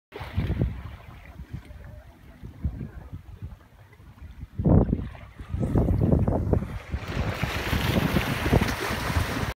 Jude casting at Hillary’s
8yr old Jude casting for Herring at Hillary’s northern rock wall.